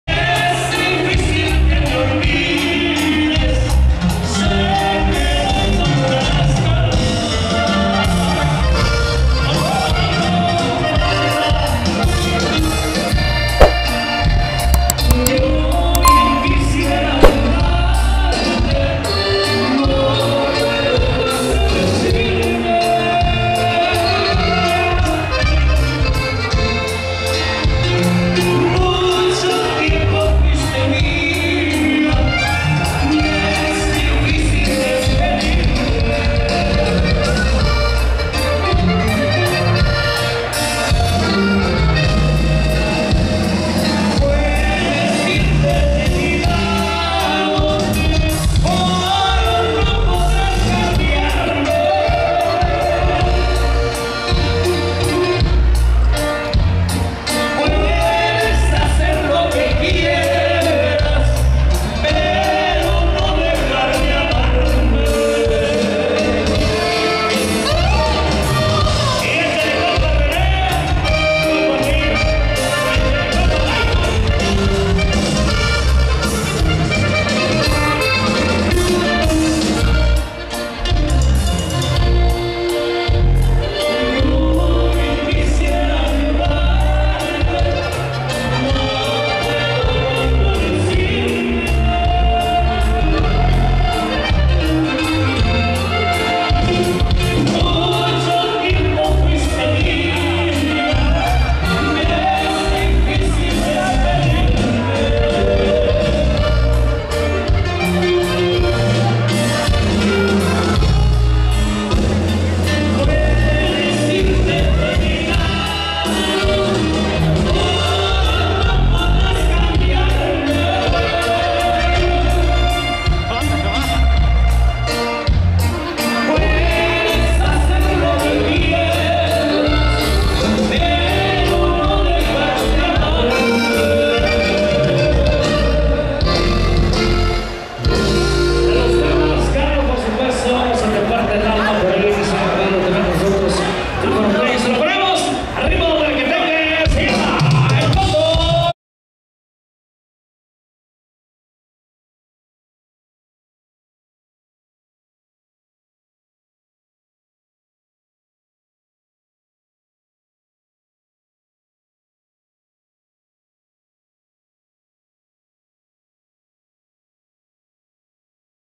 Toda una DIOSA del OLIMPO la de AMARILLO en el LIMON de PAPATZINDAN
Bienvenidos a mi canal gente bonita aqui les dejo este bailazo en el Limon de Papatzindan, Michoacan 💃🎀😍📹🎞